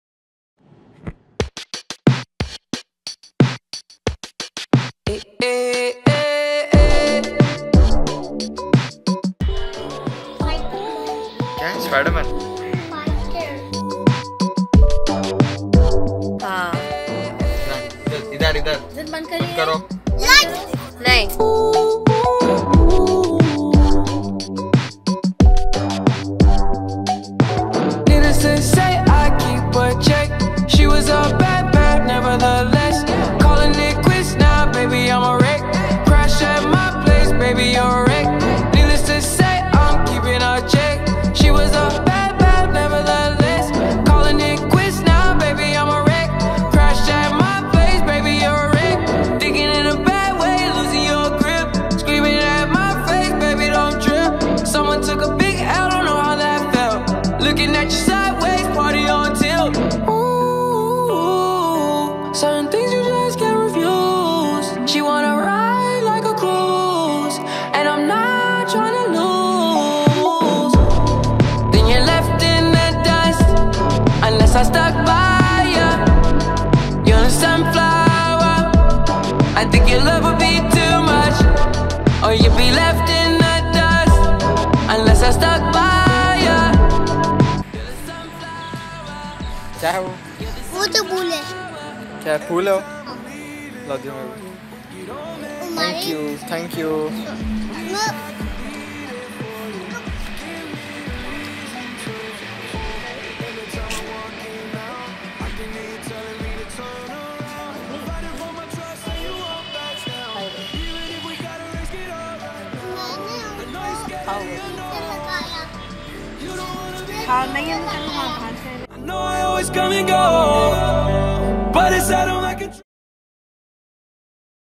THIS KID IS THE CUTEST!!!! - Zaydan Montage
When I went to india last winter.I took a bunch of videos of my naughty cousin.He is the cutest.I have compiled it and made a video.
Your daily dose of wholesome